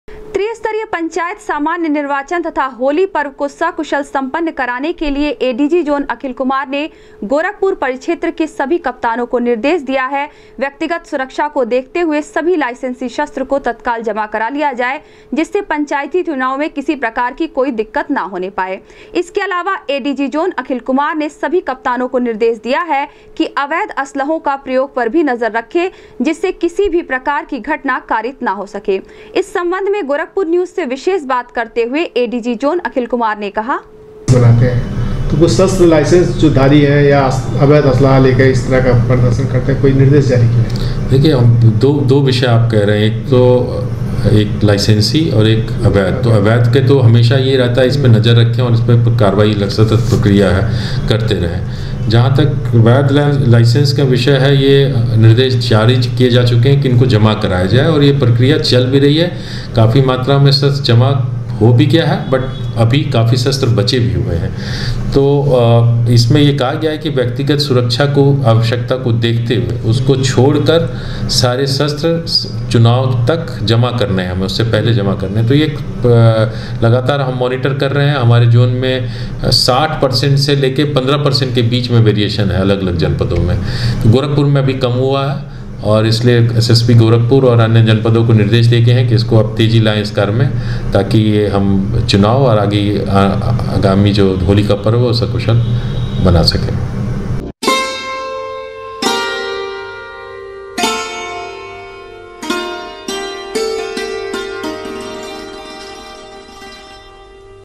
त्रिस्तरीय पंचायत सामान्य निर्वाचन तथा होली पर्व को सकुशल सम्पन्न कराने के लिए एडीजी जोन अखिल कुमार
त्रिस्तरीय पंचायत सामान्य निर्वाचन तथा होली पर्व को सकुशल सम्पन्न कराने के लिए एडीजी जोन अखिल कुमार ने गोरखपुर परिक्षेत्र के सभी कप्तानों को दिए निर्देश ,कहा,लाइसेंसी बंदूक को कराए जमा,अवैध असलहों पर भी रखें नजर